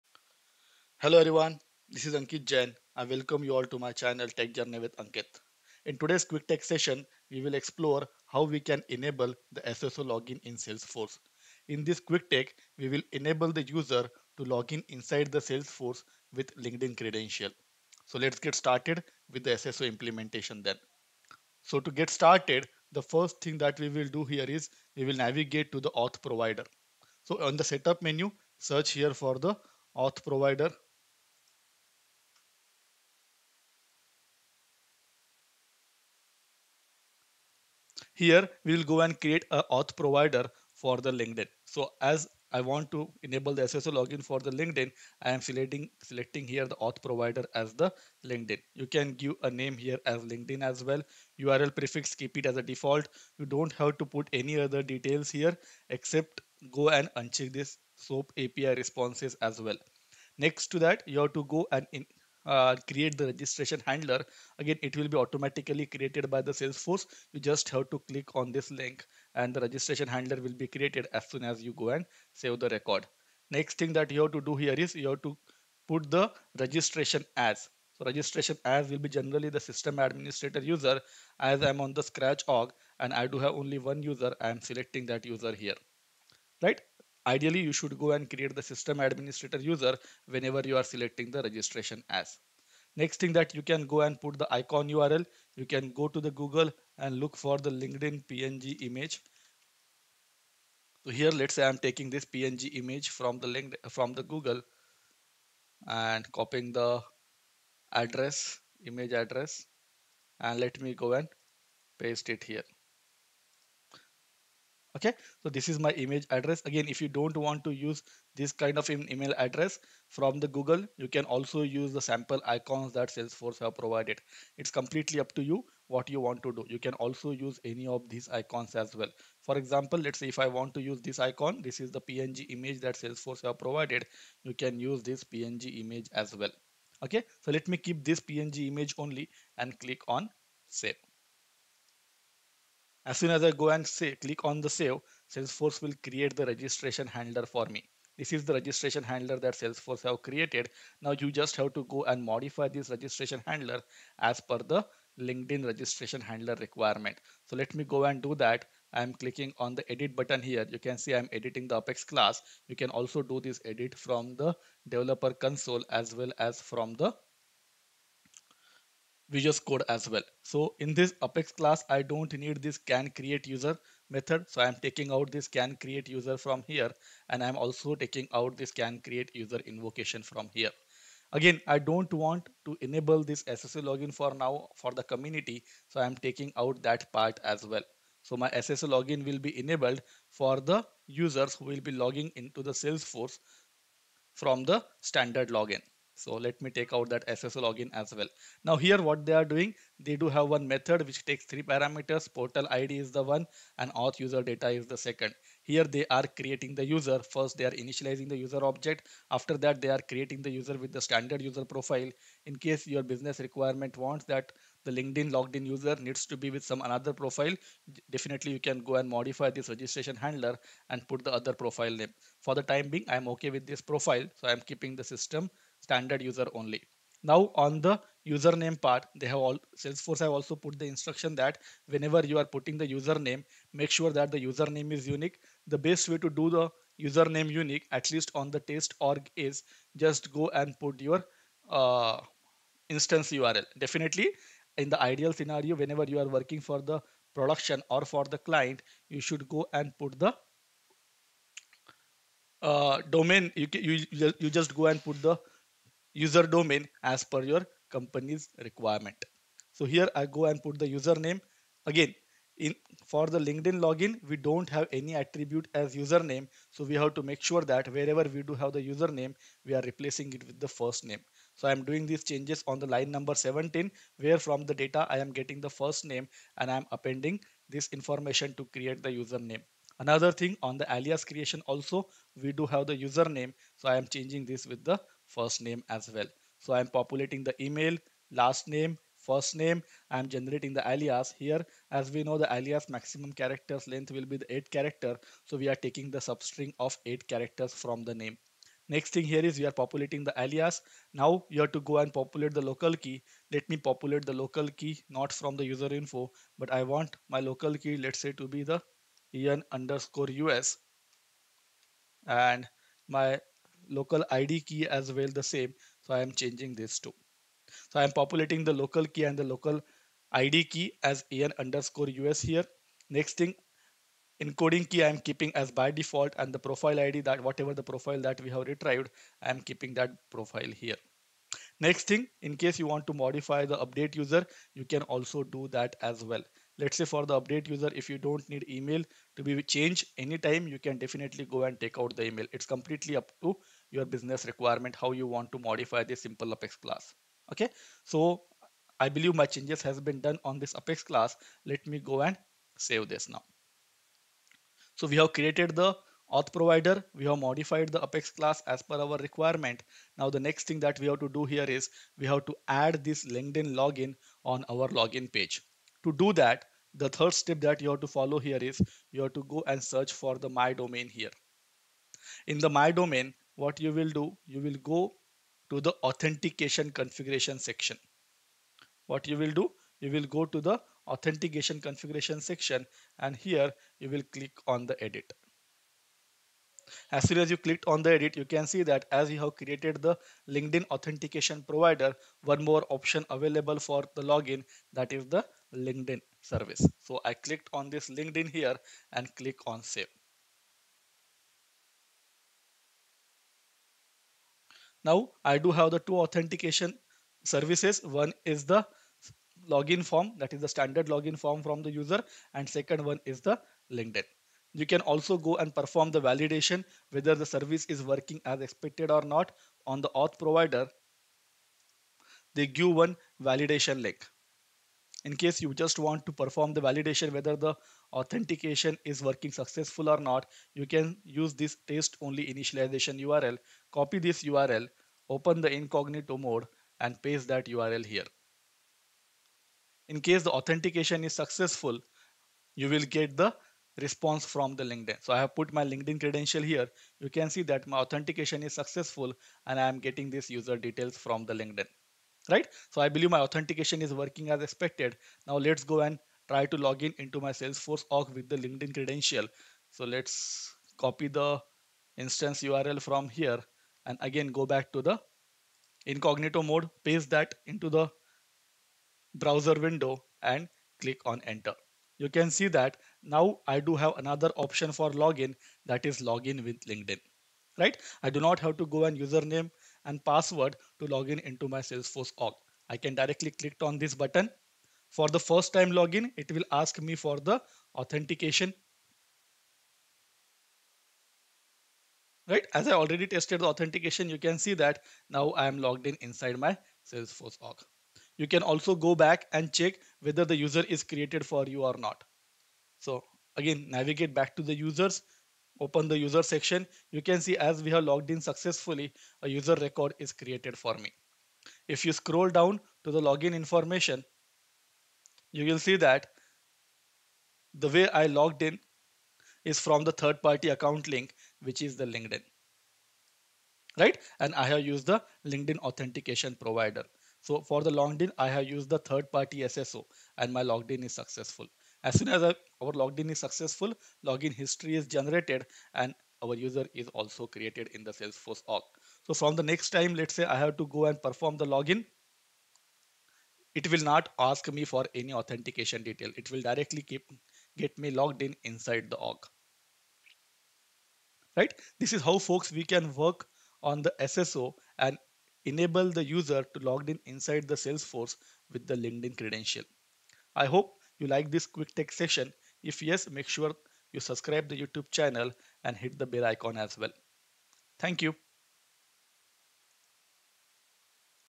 SSO Login from Salesforce With LinkedIn | Social Sign-on for Salesforce
In this power-packed tutorial, we'll cover:

🔑 The fundamentals of Single Sign-On and its importance in seamless user experiences.
🛠️ Hands-on demonstrations of setting up the SSO in a real-world scenario.
💡 Tips, tricks, and best practices for ensuring a secure and efficient SSO implementation.

Whether you're a Salesforce admin aiming to streamline user access or a developer looking to enhance user convenience, this video has something for everyone. 💼👩‍💻

Join us in unlocking the potential of SSO integration between two industry giants and revolutionize the way your users access and interact with your Salesforce platform. 🚀

Don't miss out on this invaluable opportunity to level up your Salesforce skills and create a more user-friendly experience. 📈